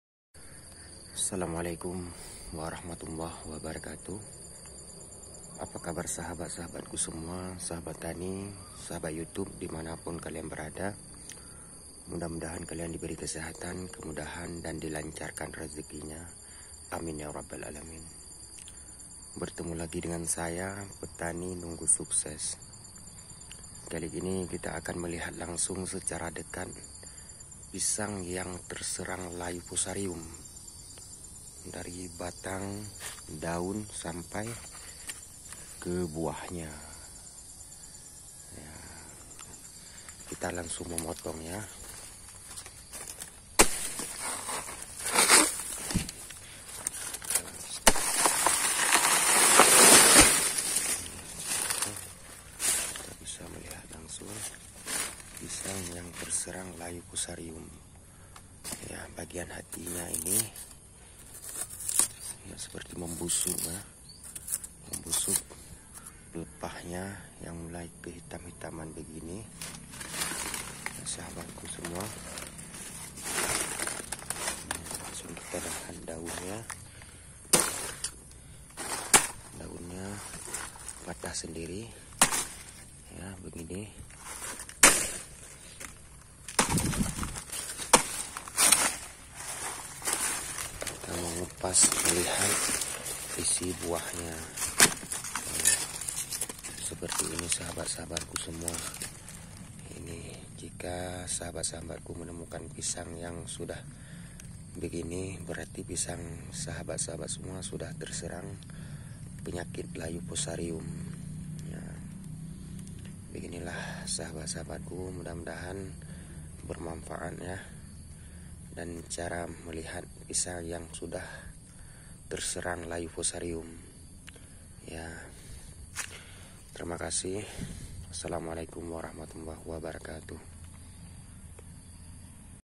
ciri ciri pisang terserang virus layu Fusarium
dari pohon pelepah yang membusuk
daunnya patah dan mengering
daging buahnya hitam dan membusuk